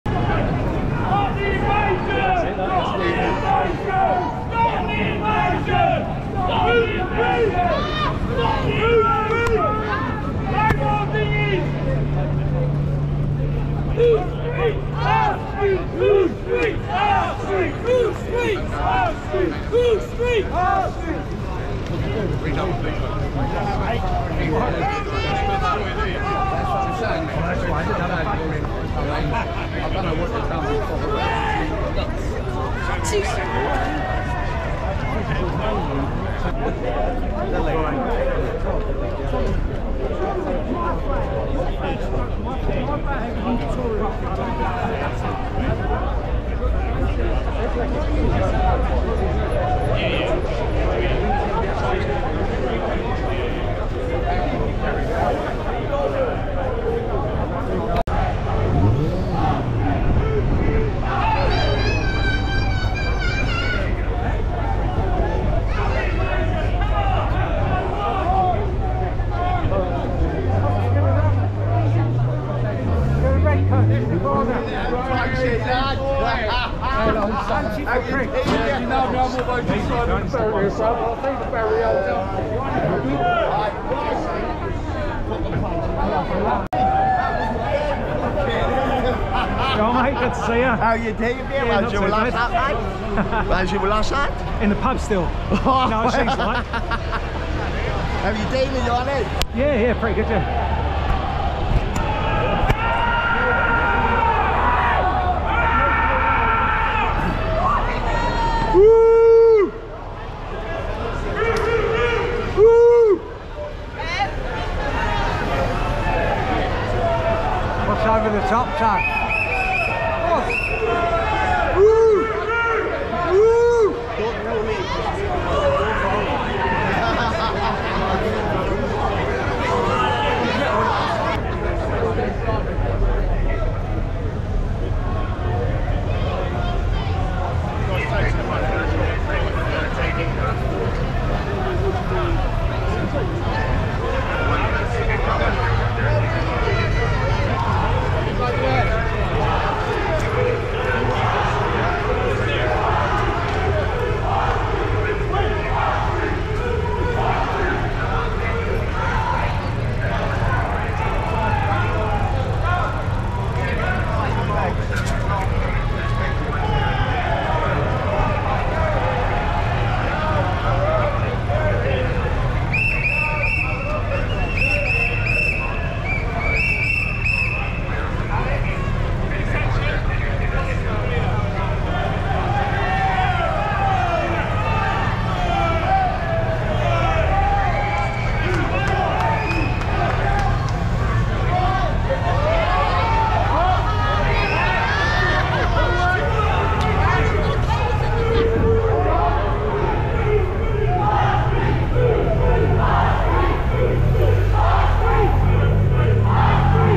Stop the Boats Demo, London 24th Sept 2022
Patriots from across the patriotic spectrum protest in London against boat loads of Illegal Immigrants arriving on the south coast.
Demonstrators marched from Parliament Square to the Home Office on Marsham Street.